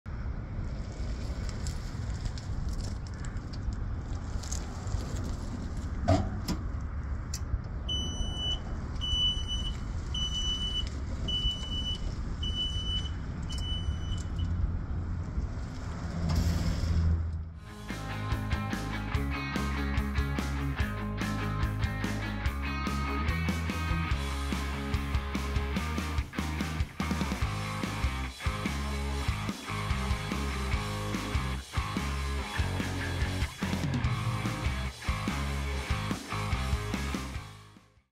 2014 Columbia BC2-L-36 Utility Cart
Watch this 2014 Columbia BC2-L-36 Utility Cart at work!
S/N: B2LE3-4WN0269
Electric
2,200LB Capacity, 76" Flatbed, Spring Suspension, 5.70-8 Tires, *Non-Titled
Hours: 831